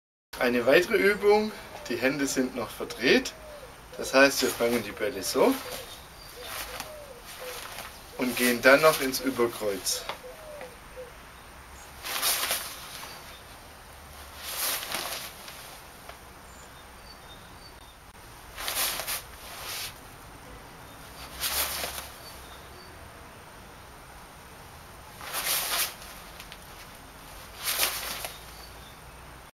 Übung 7: Arm-Arm-Koordination mit Ball
Die Hände sind entgegengesetzt und fangen den Ball über Kreuz